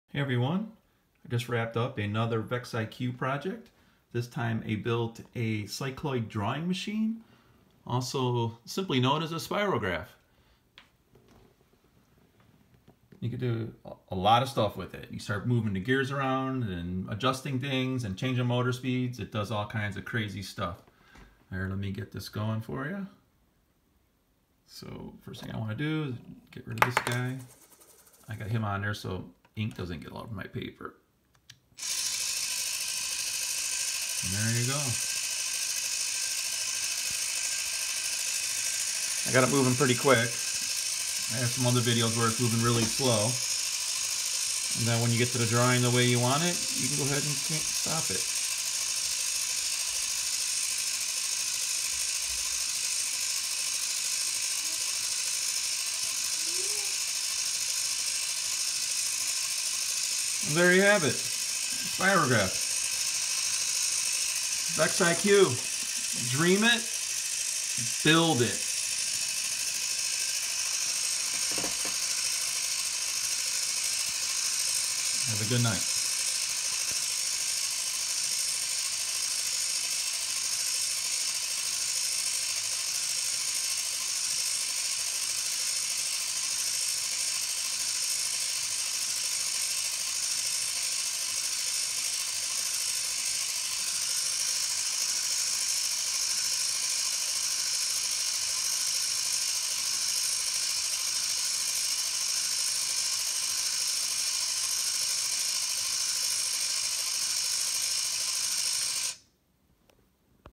VEX IQ Cycloid Drawing Machine v1
I was playing with the VEX IQ gears just like the old school Spirograph kits and thought it would be fun to add some motors. Did a little research and found some other creative hackers with the same idea. Here's my version all done with VEX IQ and the VEX Brain unit.